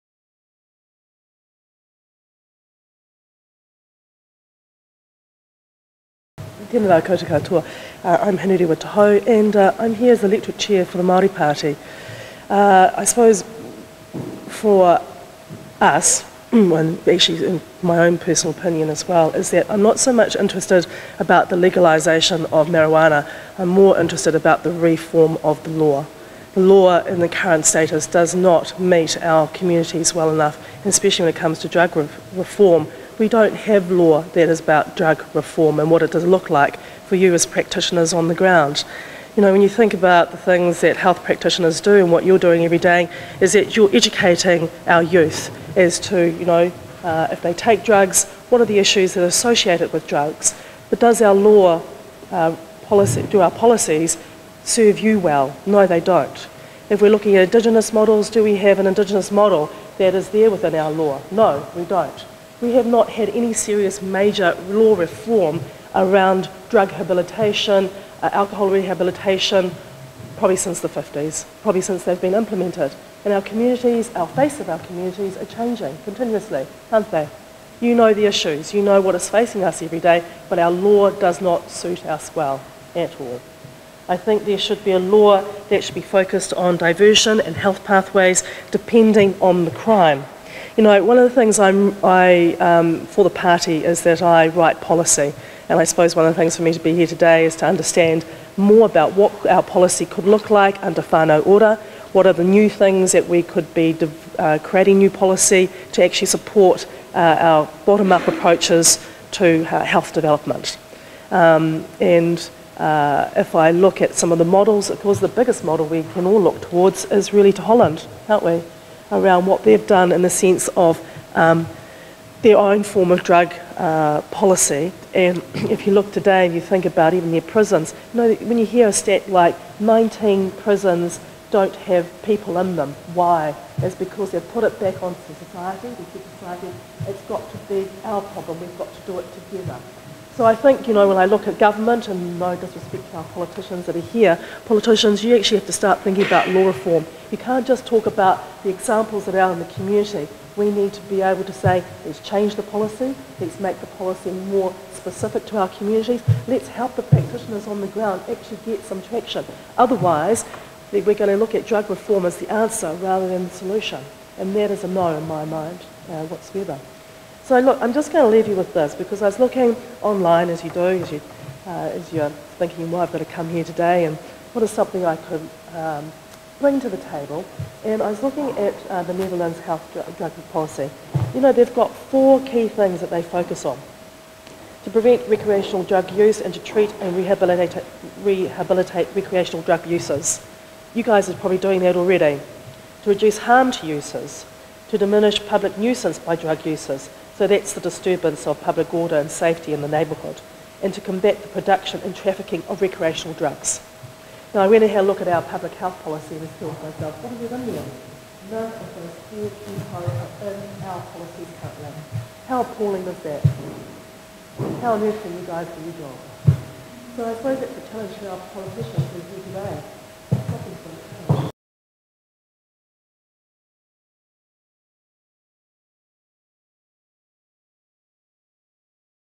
Northland Hui: Call for action on drug law harm, Part 11 - Hinurewa te Hau
Hinurewa te Hau, Tai Tokerau Electorate Chair for the Māori Party speaks at a Northland public meeting in Kaikohe on 19 November 2016. This was the first of a series of hui about shifting to better drug law for Māori. There was recognition about the need to reduce harm caused by drugs in their communities, and the damage caused by current laws. National, Labour, Green and NZ First MPs all expressed support for health-focused interventions and some agreement that laws must change to support Māori.